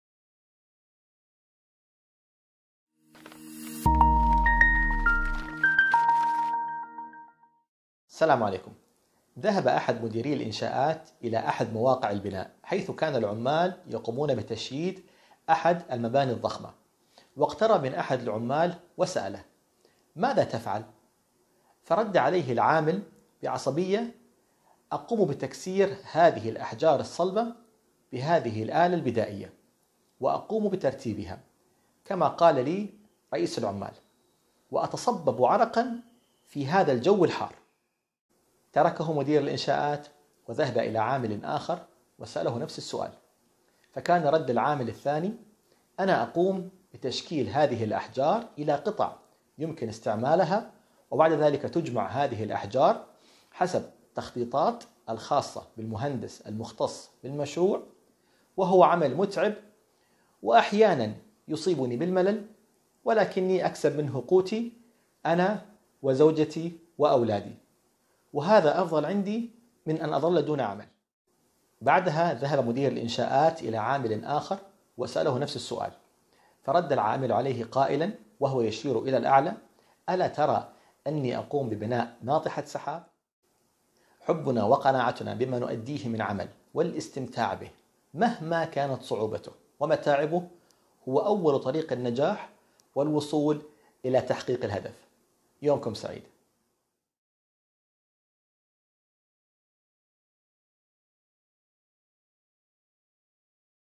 هل عملك شاق اوصعب ؟ هل تستمتع بعملك ؟ #
Video from Hani Alzubaidi

هل عملك شاق اوصعب ؟
هل تستمتع بعملك ؟
قصة جميلة عن الاستماع بالعمل الذي نقوم به,, فهذا اول طريق النجاح